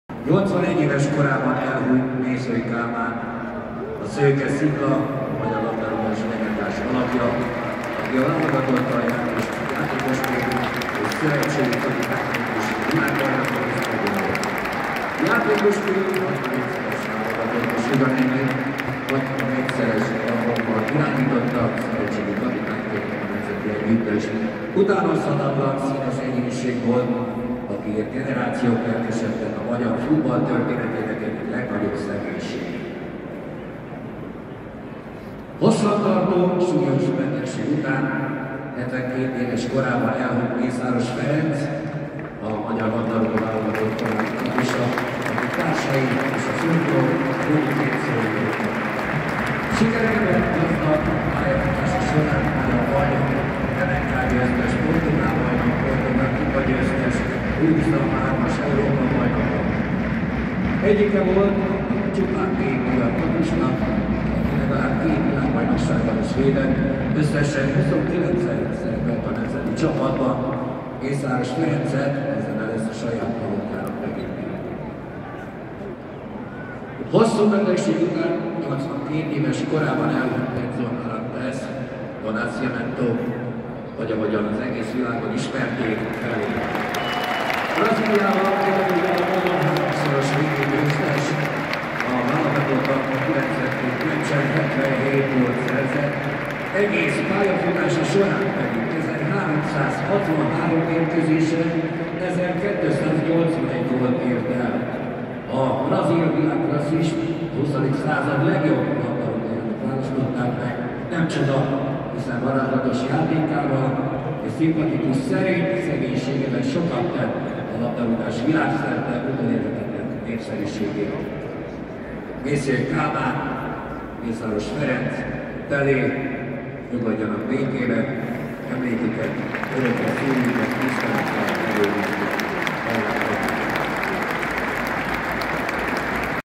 FTC-Vasas: vastaps Mészöly Kálmánnak, Mészáros Ferencnek és Pelének
Legendákra emlékeztek a kezdés előtt, ráadásul méltóképpen!